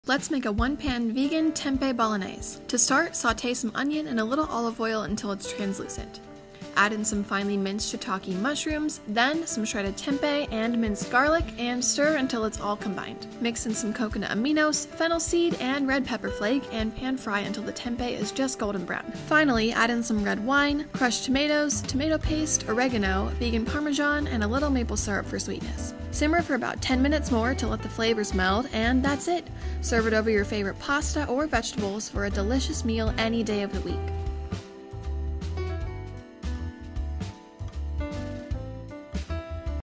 1-Pan Vegan Tempeh Bolognese | Minimalist Baker Recipes
Our 1-Pan Tempeh Bolognese is a HEARTY, super easy, and oh-so-versatile dish you have to make this week! Pair it with pastas, eggplant parmesan,  lasagna, and MORE for a delicious, flavorful, plant-based meal!